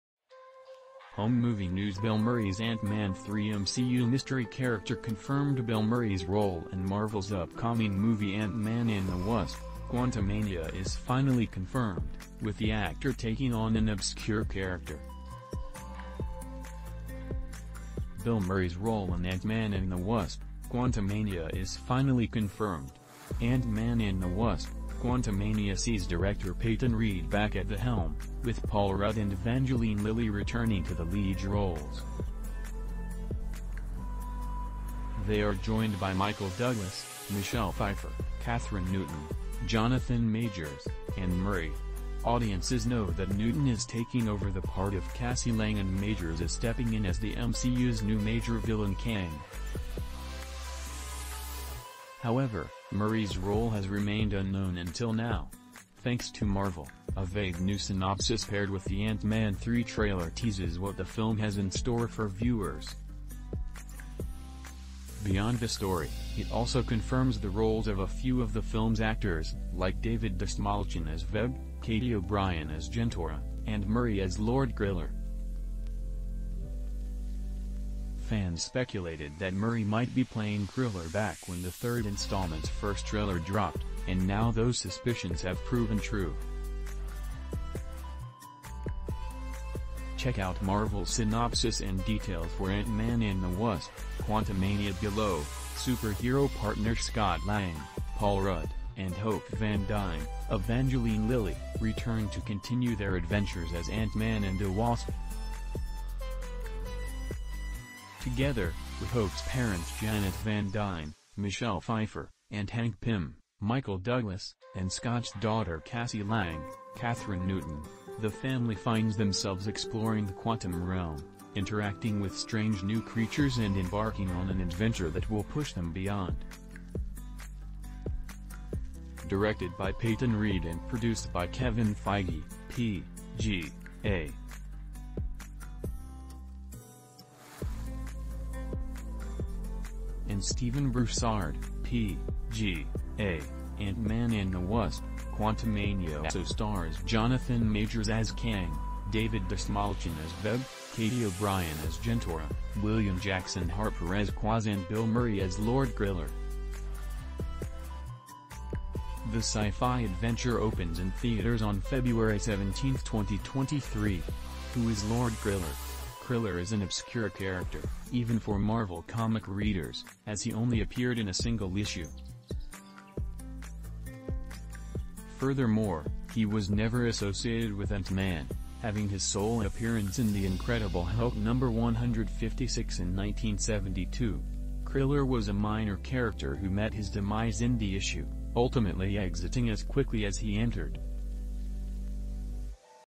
Bill Murray's Ant-Man 3 MCU Mystery Character Confirmed
Home Movie News Bill Murray's Ant-Man 3 MCU Mystery Character ConfirmedBill Murray’s role in Marvel’s upcoming movie Ant-Man and the Wasp: Quantumania is finally confirmed, with the actor taking on an obscure character. Bill Murray's role in Ant-Man and the Wasp: Quantumania is finally confirmed. Ant-Man and the Wasp: Quantumania sees director Peyton Reed back at the helm, with Paul Rudd and Evangeline Lilly returning to the lead roles. They are joined by Michael Douglas, Michelle Pfeiffer, Kathryn Newton, Jonathan Majors, and Murray. Audiences know that Newton is taking over the part of Cassie Lang and Majors is stepping in as the MCU's new major villain Kang. However, Murray's role has remained unknown until now. Thanks to Marvel, a vague new synopsis paired with the Ant-Man 3 trailer teases what the film has in store for viewers. Beyond the story, it also confirms the roles of a few of the film's actors, like David Dastmalchian as Veb, Katy O'Brian as Jentorra, and Murray as Lord Krylar. Fans speculated that Murray might be playing Krylar back when the third installment's first trailer dropped, and now those suspicions have proven true. Check out Marvel's synopsis and details for Ant-Man and the Wasp: Quantumania below:"Super-Hero partners Scott Lang (Paul Rudd) and Hope van Dyne (Evangeline Lilly) return to continue their adventures as Ant-Man and the Wasp. Together, with Hope’s parents Janet van Dyne (Michelle Pfeiffer) and Hank Pym (Michael Douglas), and Scott’s daughter Cassie Lang (Kathryn Newton), the family finds themselves exploring the Quantum Realm, interacting with strange new creatures and embarking on an adventure that will push them beyond the limits of what they thought possible. Directed by Peyton Reed and produced by Kevin Feige, p.g.a. and Stephen Broussard, p.g.a., “Ant-Man and The Wasp: Quantumania” also stars Jonathan Majors as Kang, David Dastmalchian as Veb, Katy O'Brian as Jentorra, William Jackson Harper as Quaz and Bill Murray as Lord Krylar. The sci-fi adventure opens in theaters on Feb. 17, 2023."Who Is Lord Krylar? Krylar is an obscure character, even for Marvel comic readers, as he only appeared in a single issue. Furthermore, he was never associated with Ant-Man, having his sole appearance in The Incredible Hulk #156 in 1972. Krylar was a minor character who met his demise in the issue, ultimately exiting as quickly as he entered. The story in The Incredible Hulk #156 sees Hulk shrink down to enter the Micro-Verse, where he is returning to visit a love interest, Jarella, on the small planet of K'ai. Instead, Hulk finds K'ai destroyed, with Jarella kidnapped by the warlord Visis. Krylar is introduced as a scientist working for Visis who creates a machine that can physically manifest someone's deepest fears, with his machine creating an out-of-control Hulk. How Lord Krylar Could Factor Into Ant-Man 3's PlotLord Krylar's inclusion in Ant-Man 3 likely boils down to his existence inside the comic's Micro-Verse.